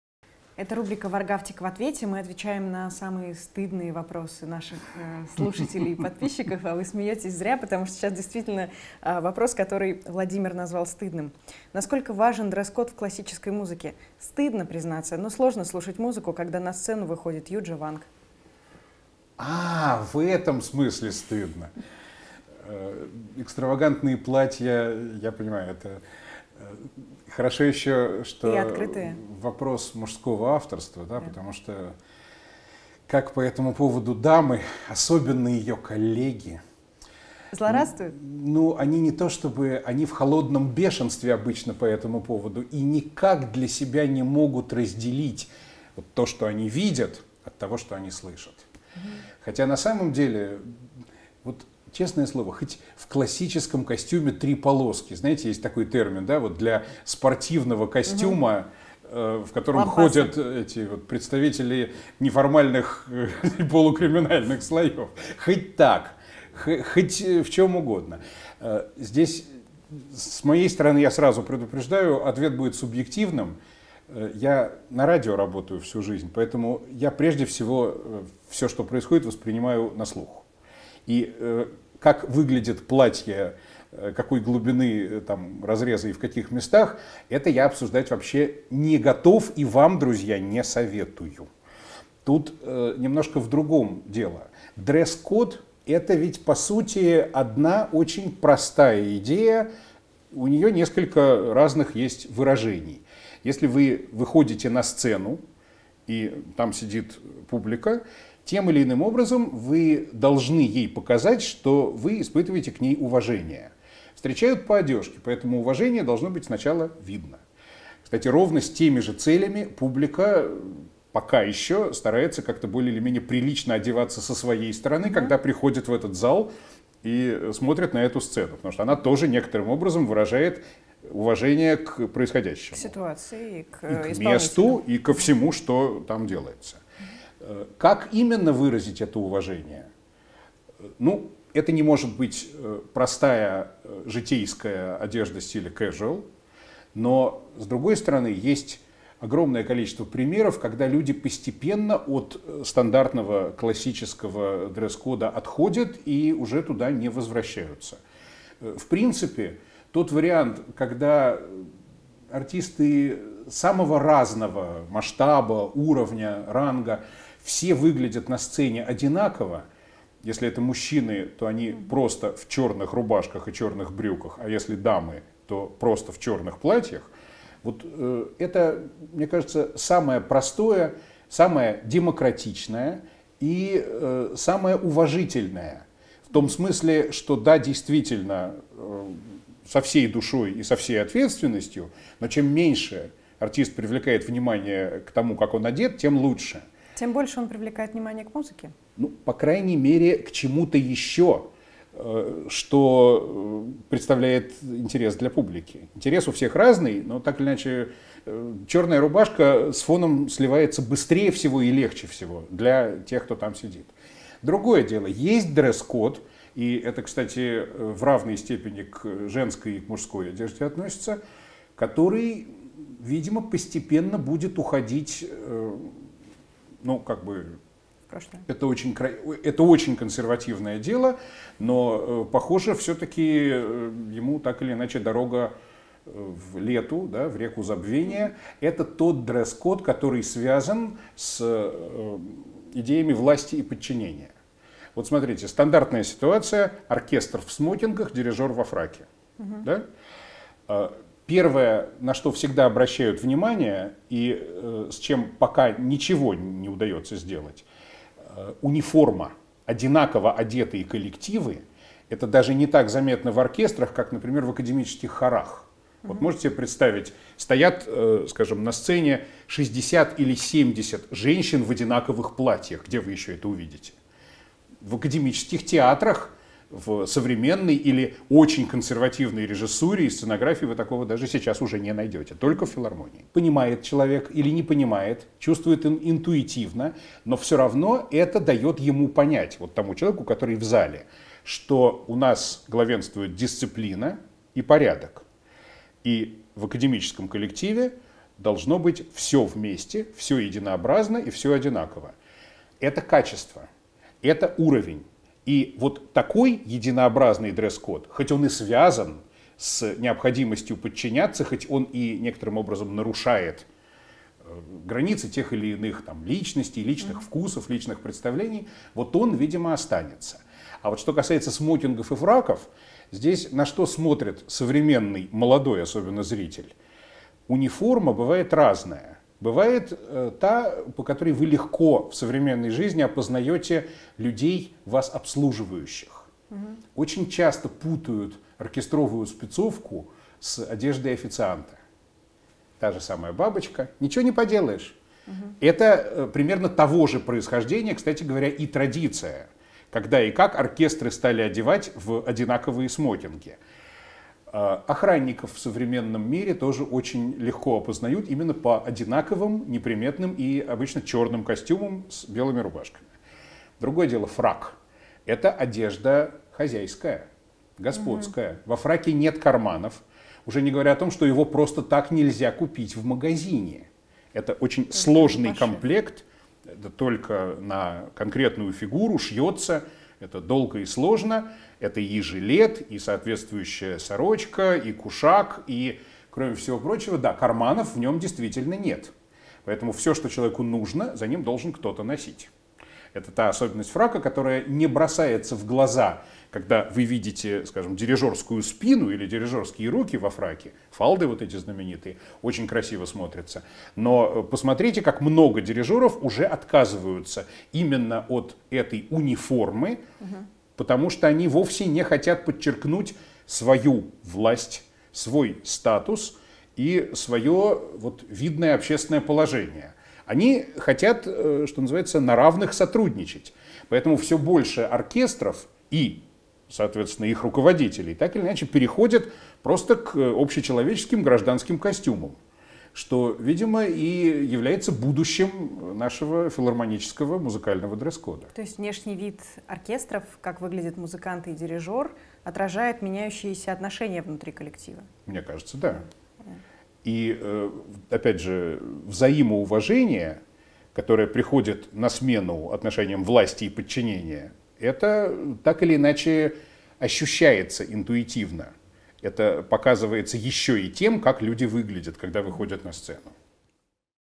«Варгафтик в ответе» про дресс-код в классической музыке
Можно ли выступать на академической сцене в откровенных платьях? Почему все оркестранты выглядят одинаково? С чем ассоциируется фрак дирижера? Рассуждаем с Артёмом Варгафтиком о дресс-коде, принятом в мире классической музыки.